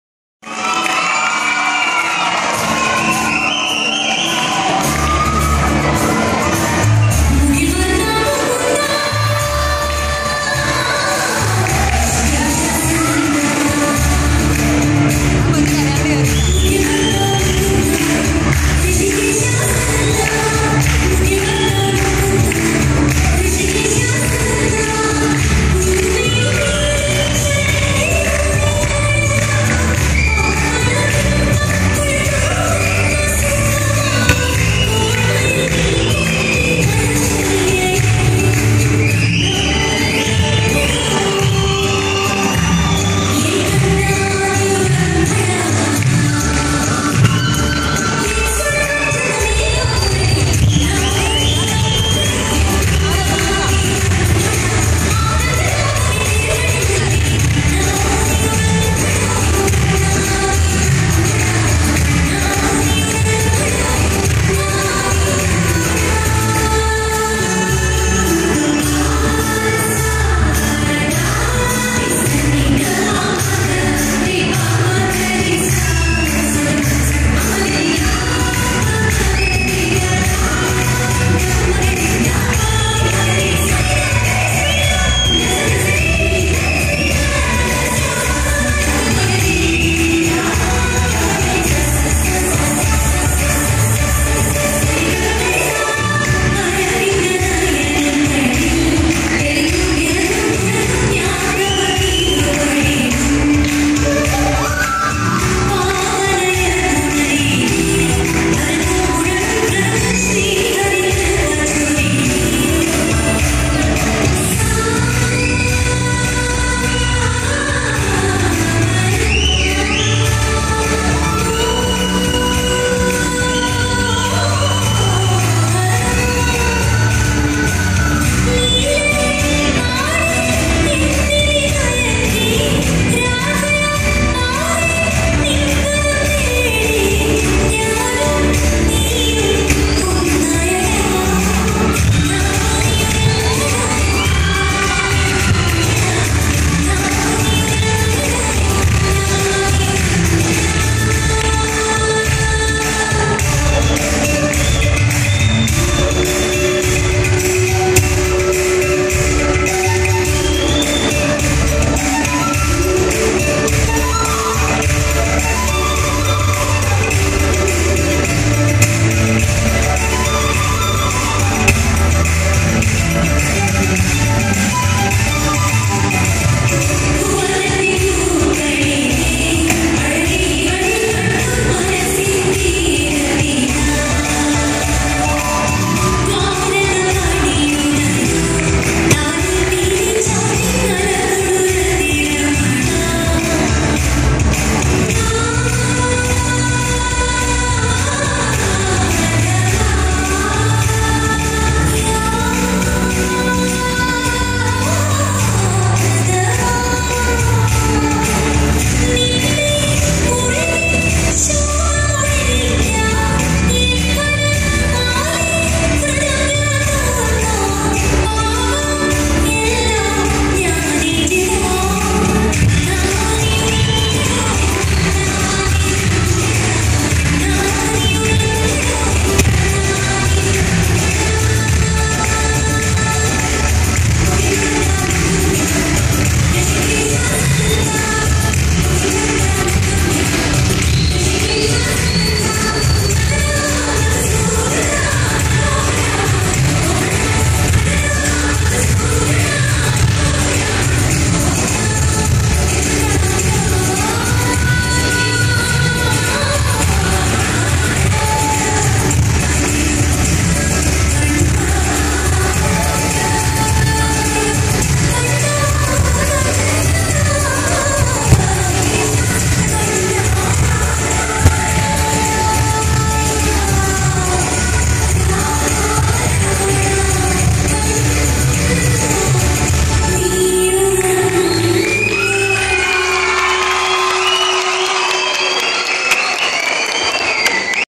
Dance by students of Govt. Girls School, Thripunithura at St. Ignatius VHSS, Kanjiramattom
This is not a competition. This dance is performed as part of the inaugural function of Thripunithura Sub district Kalolsavam.